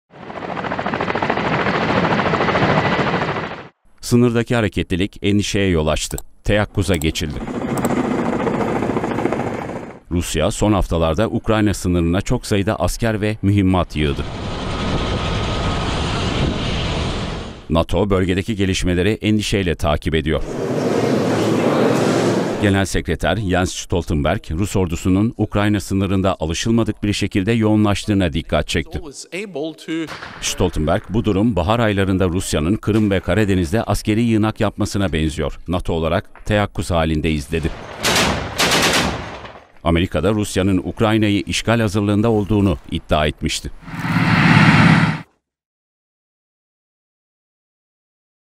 Ukrayna sınırındaki hareketlilik NATO'da endişe yarattı! "Teyakkuz hâlindeyiz"
NATO, Rusya'nın Ukrayna sınırına asker göndermesiyle ilgili değerlendirmede bulundu. "Teyakkuz halindeyiz" dedi.

TÜRKSAT 4A
Frekans: 12729
Sembol: 30000
Polarizasyon: V (Dikey)